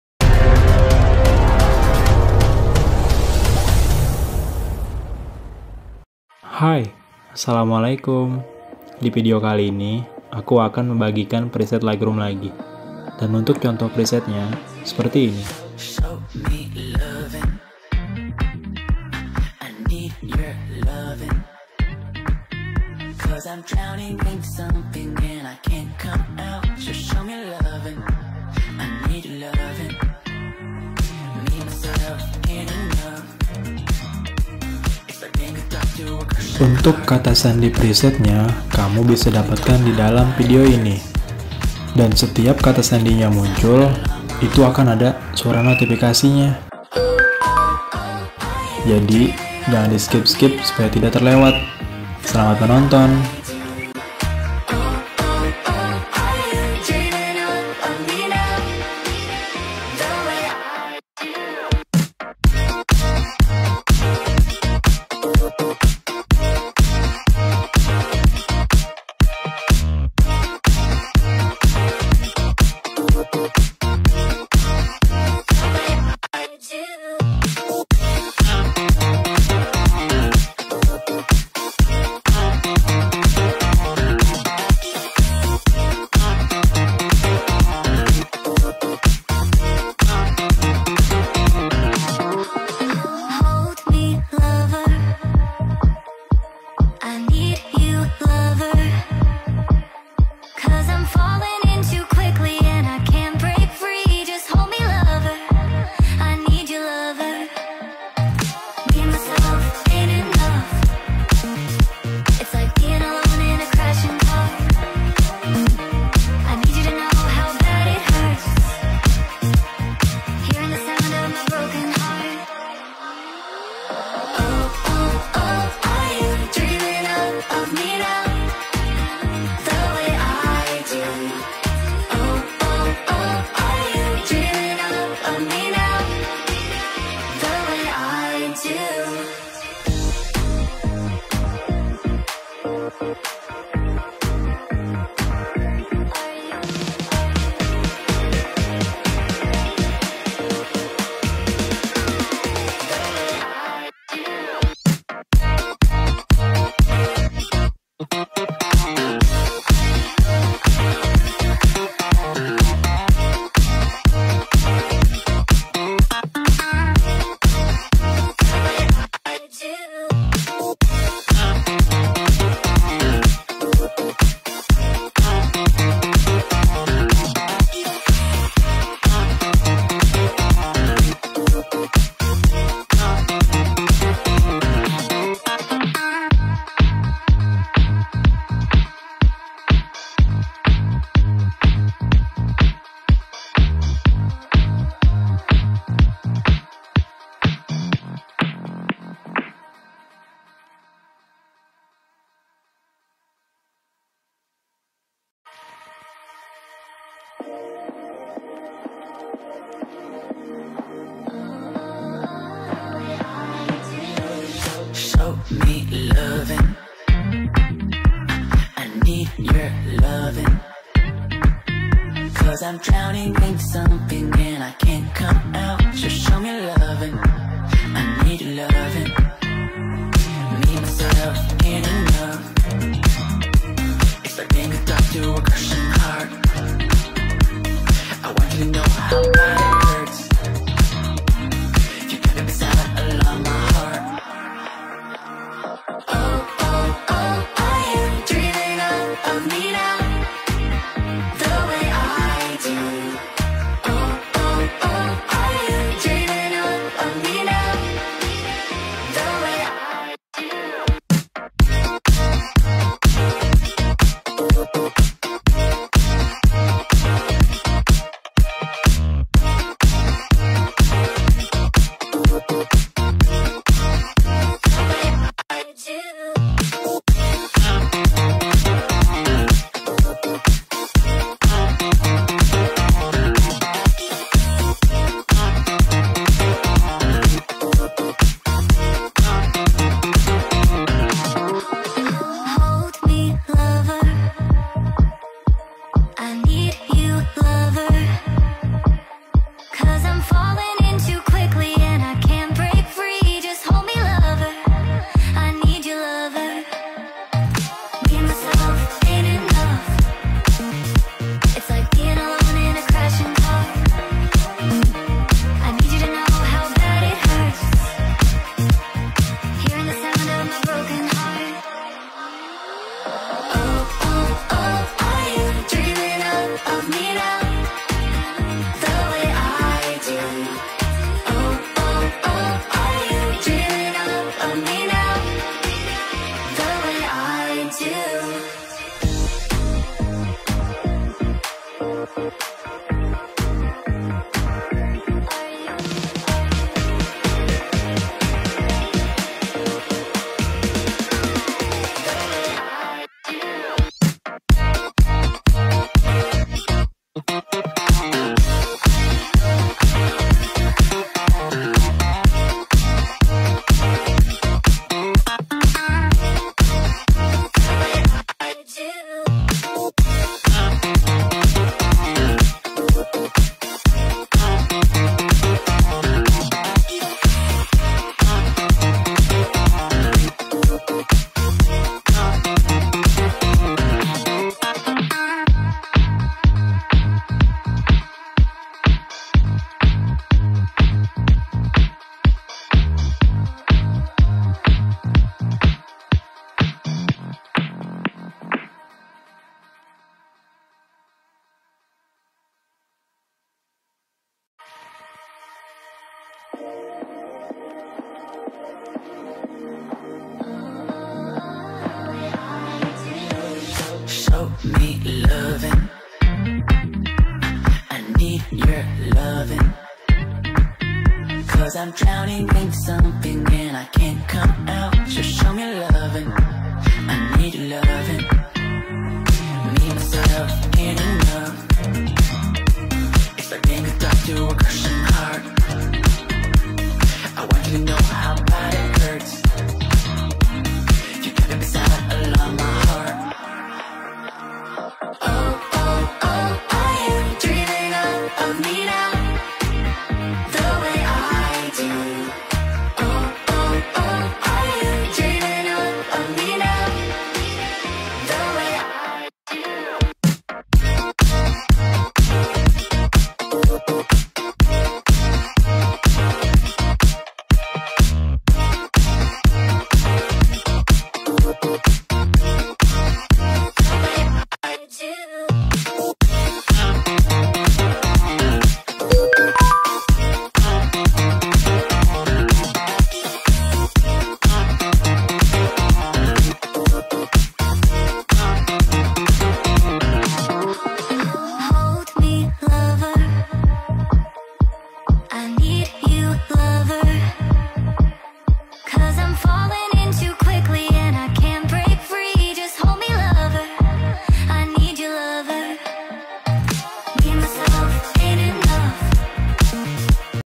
50+ PRESET LIGHTROOM | SPESIAL EFEK IPHONE V2 | LIGHTROOM TUTORIAL
50+ PRESET LIGHTROOM | SPESIAL EFEK IPHONE V2 | LIGHTROOM TUTORIAL

Preset Lightroom yang akan aku bagikan di video kali ini adalah update terbaru Preset Lightroom Spesial Efek iPhone V2.

Cara Mendapatkan Password :
📌 Password akan muncul di bagian "POJOK KANAN ATAS" video
📌 Cukup satu kali nonton dari awal sampai akhir jangan di skip, Karena nanti akan ada suara notifikasi ketika password muncul
📌 Password terdiri dari 4 digit angka, dan akan muncul secara terpisah (dipisahkan menjadi dua bagian)

Sebagai contoh:
Dalam contoh di atas, password nya adalah 1234.

Akses untuk mengunduh preset :
Preset Untuk Pengguna Android (50 Preset XMP)
🔻 Link Passdropit :
Preset untuk pengguna IPhone (10 Preset DNG)
🔻 Link MediaFire :
Gunakan Google Chrome untuk mengunduh Preset.

Gambar : Pinterest
🔻Semua gambar yang ditampilkan dalam video, adalah milik pemiliknya masing-masing. Semua foto hanya untuk tujuan ilustrasi.